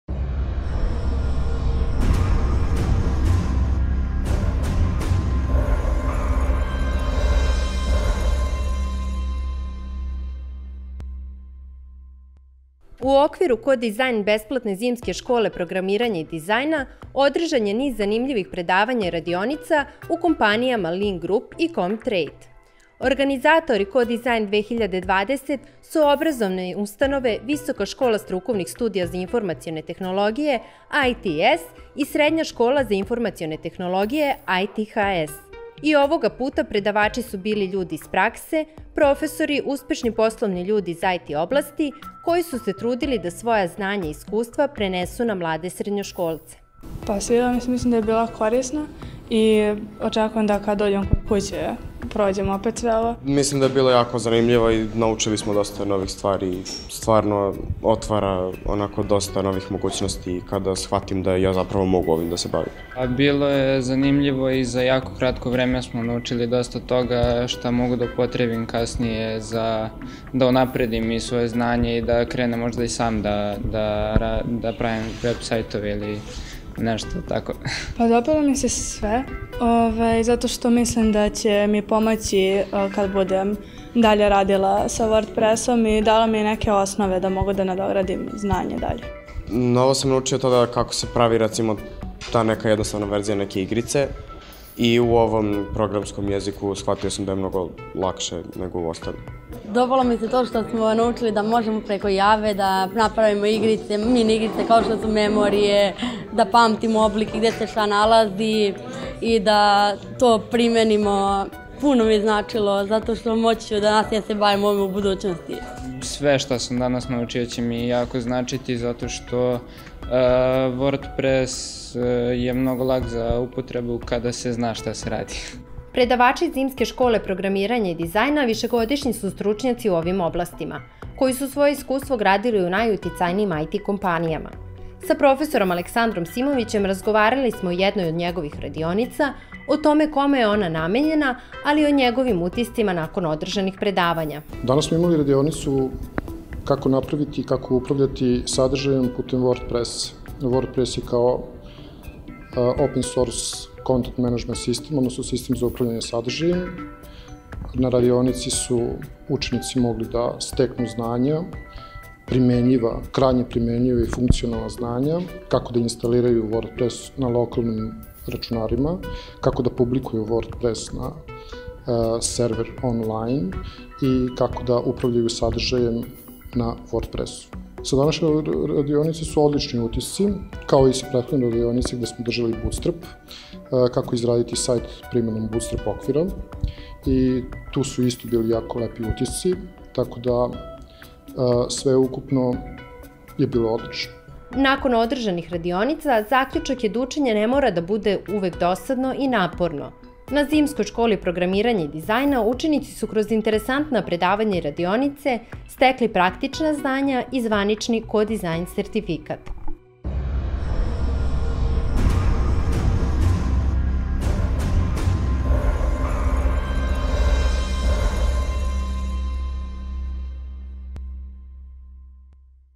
CoDEsign2020 – Zimska škola programiranja i dizajna
Zimska škola programiranja i dizajna – coDEsign2020 trajala je od 3. do 9. februara u organizaciji Visoke škole strukovnih studija za informacione tehnologije – ITS i Srednje škole za informacione tehnologije – ITHS. Polaznici su imali priliku da slušaju zanimljiva predavanja i radionice sa kojih su poneli korisna znanja iz oblasti informacionih tehnologija. Kao i prethodnih godina, kabineti i amfiteatri bili su puni radoznalih učenika.